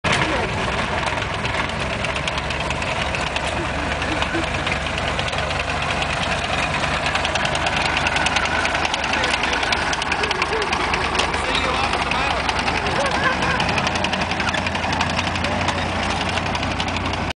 German Halftrack driving - See You After Paris -
German Half Track at Fort Lytton , Brisbane Australia 2005.
- See you after Paris - Tank driving running operating operational